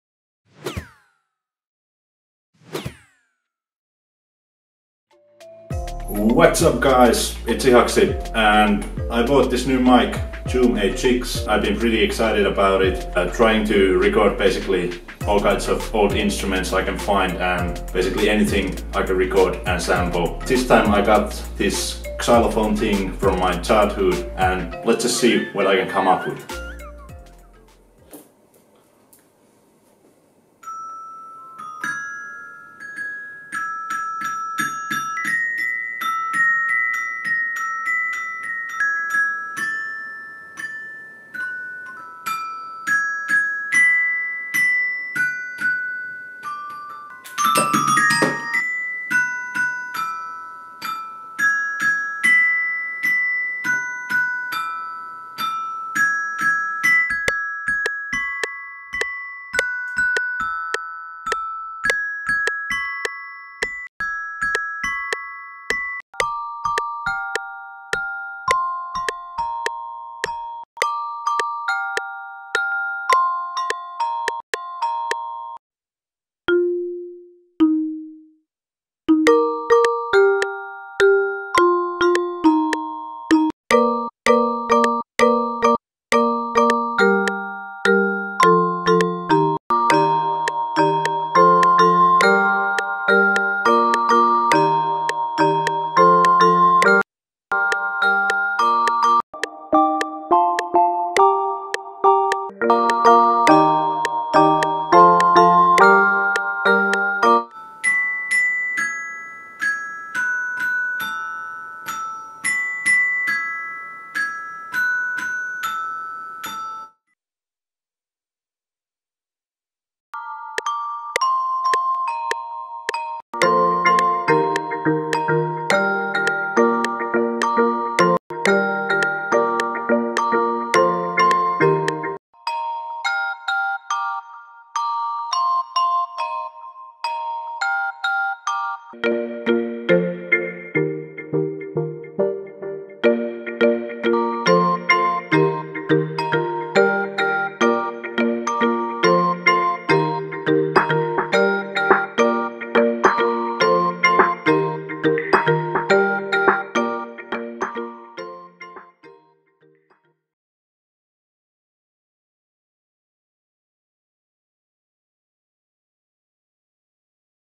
Making a Beat - New Mic and Sampling Xylophone (Producing Hip Hop / Pop / Trap in FL Studio)
So I bough a new mic Zoom H6 and been recording all kinds of stuff. Thought I'd share a short clip of the process of a simple recording turning into a beat. Let me know if you're interested in seeing more this type of stuff in the future!

The beat will published later this evening!

Cheers!
Ihaksi
Suomi Finland